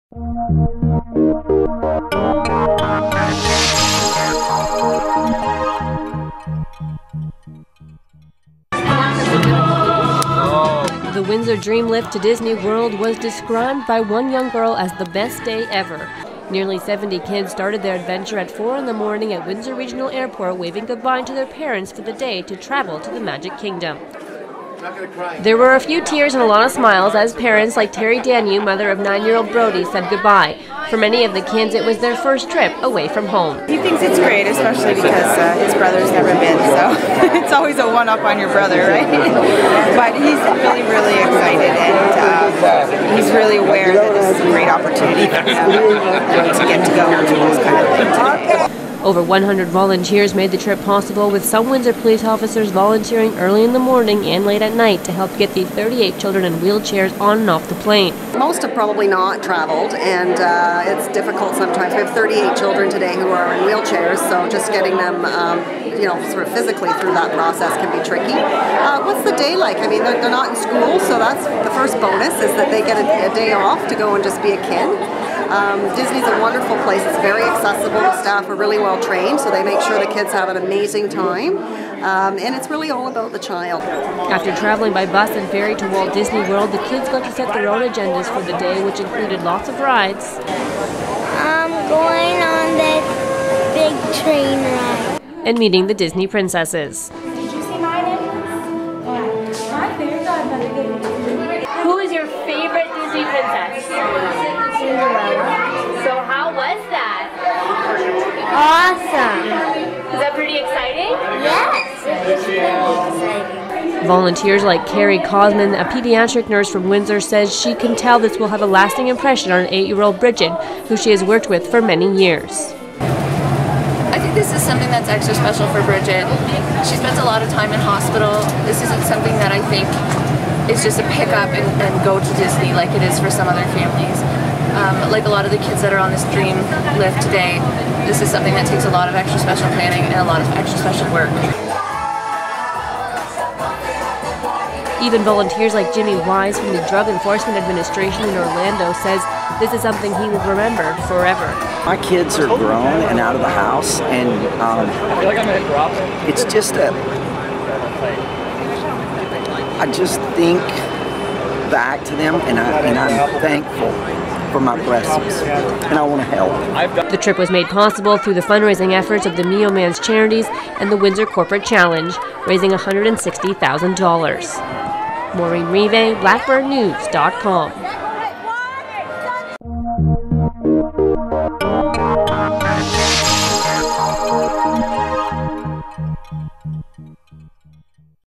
Windsor DreamLift 'best day ever'
The Sunshine Foundation sent nearly 70 local children to Walt Disney World through Operation DreamLift